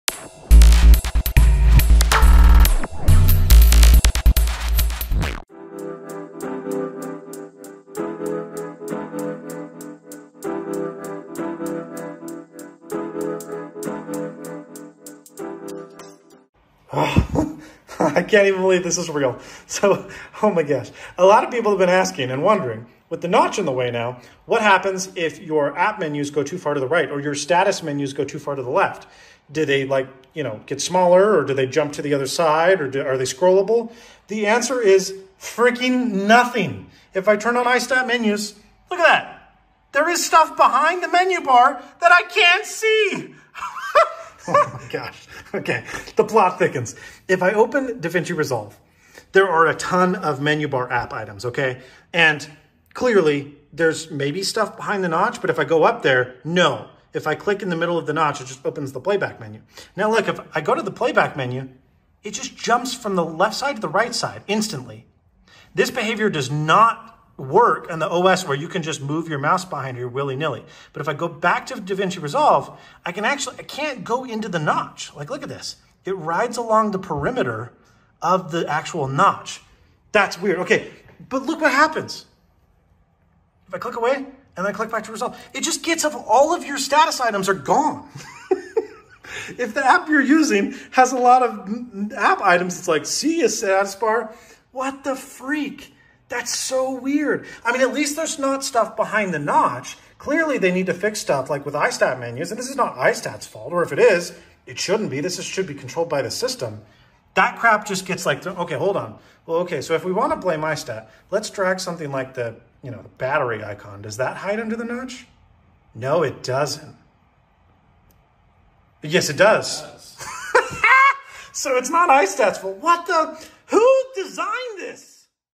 Apple: "The notch is a smart way to display more content on the MacBook Pro"
The MacBook Pro notch, ladies and gentlemen. HOW IS THIS SHIPPABLE? WHAT IS THIS?! WHO DESIGNED THIS?! 😂

- Apple: We have beautifully designed a new MacBook with a notch to avoid bezels.
- People: But it's so awful!
- Apple: You can conceal it by putting a black bar on top!
- People: ...and getting a bezel!